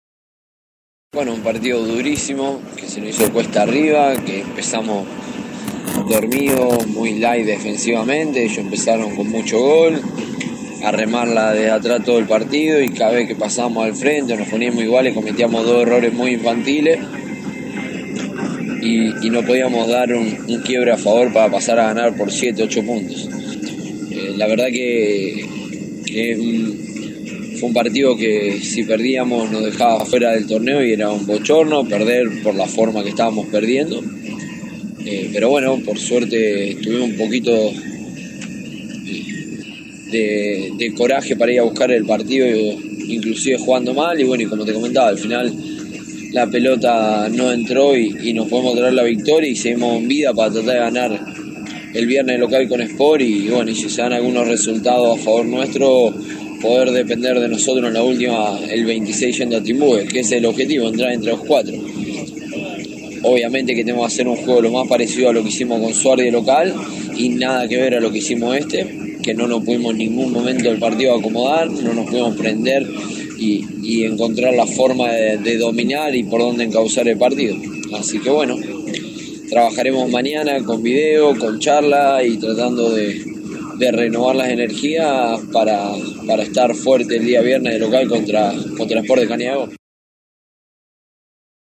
BÁSQUET FEDERAL: TRIUNFO AGÓNICO DE BELL EN SUNCHALES
En un final para el infarto el equipo de Agustìn Pujol venciò 85 a 84 a Libertad y pelea por alcanzar la clasificaciòn a la pròxima fase del torneo federal de básquet.